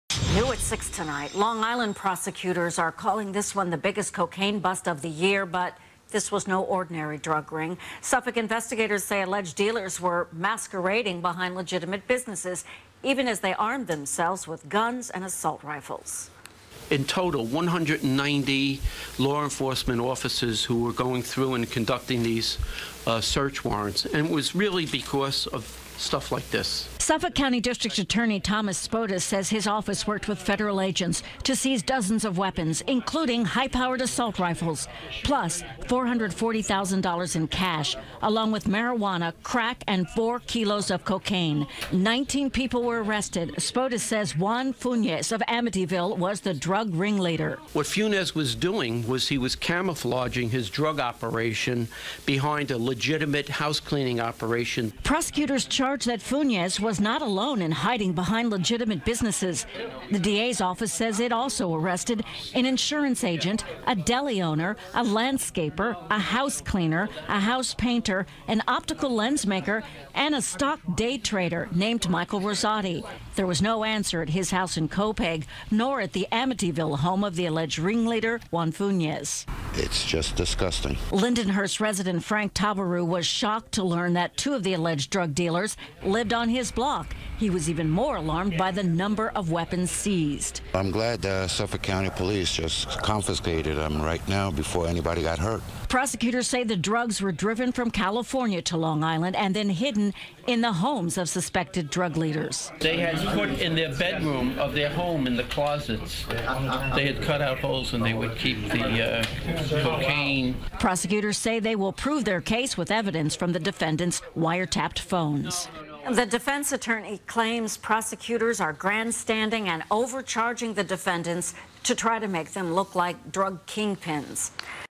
Prosecutors: Long Island Drug Dealers Hid Behind Legitimate Businesses
As CBS 2's Dana Tyler reported, Long Island prosecutors are calling it the biggest cocaine bust of the year.